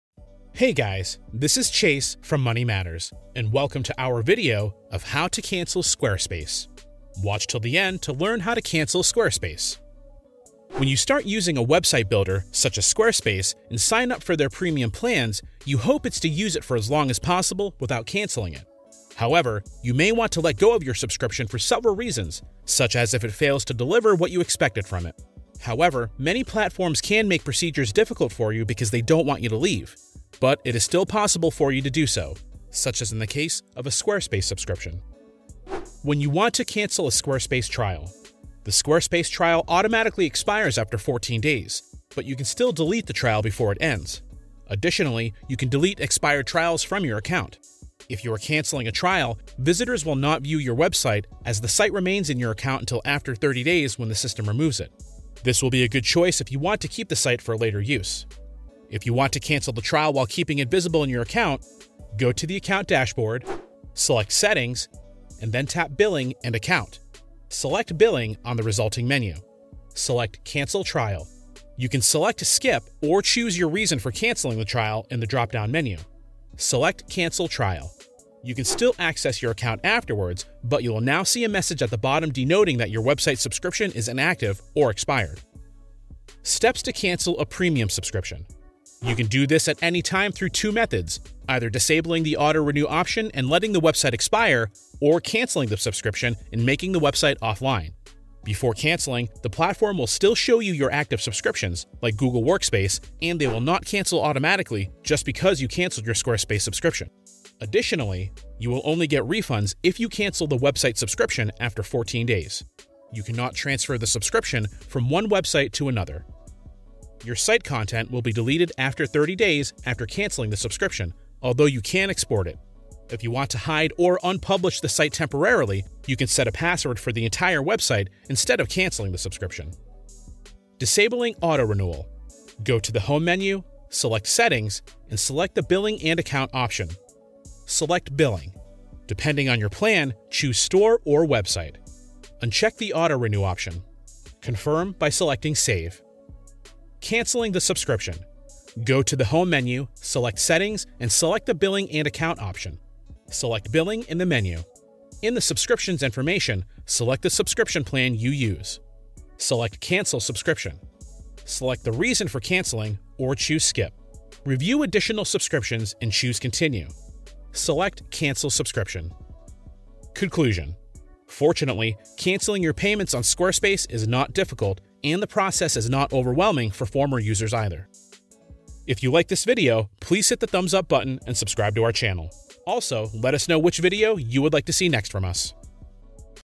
How to cancel Squarespace
When you start using a website builder such as Squarespace and sign up for their premium plans, your hope is to use it for as long as possible without cancelling it. However, you may want to let go of your subscription for several reasons, such as if it fails to deliver what you expected from it.
However, many platforms can make procedures difficult for you because they do not want you to leave, but it is still possible for you to do so, such as in the case of a Squarespace subscription.
When you want to cancel a Squarespace trial
The Squarespace trial automatically expires after 14 days, but you can still delete the trial before it ends. Additionally, you can delete expired trials from your account.
If you are cancelling a trial, visitors will not view your website as the site remains in your account until after 30 days when the system removes it. This will be a good choice if you want to keep the site for later use.
If you want to cancel the trial while keeping it visible in your account:
Go to the Account Dashboard.
Select Settings, and then tap Billing and Account.
Select Billing on the resulting menu.
Select Cancel Trial.
You can select Skip, or choose your reason for cancelling the trial in the drop-down menu.
Select Cancel Trial.
You can still access your account afterwards, but you will now see a message at the bottom denoting that your website subscription is inactive or expired.

Steps to cancel a premium subscription
You can do this at any time through two methods: either disabling the auto-renew option and letting the website expire, or canceling the subscription and making the website offline. Before cancelling, the platform will still show you your active subscriptions like Google Workspace, and they will not cancel automatically just because you cancelled your Squarespace subscription.
Additionally:
You will only get refunds if you cancel the website subscription after 14 days
You cannot transfer the subscription from one website to another
Your site content will be deleted after 30 days after cancelling the subscription, although you can export it
If you want to hide or un-publish the site temporarily, you can set a password for the entire website instead of cancelling the subscription.
Disabling auto-renewal
Go to the Home menu, select Settings, and select the Billing and Account option.
Select Billing.
Depending on your plan, choose Store or Website.
Uncheck the Auto-Renew option.
Confirm by selecting Save.

Cancelling the subscription
Select Billing in the menu.
In the Subscriptions information, select the Subscription plan you use.
Select Cancel Subscription
Select the reason for cancelling, or choose Skip.
Review additional subscriptions and choose Continue.
Select Cancel Subscription.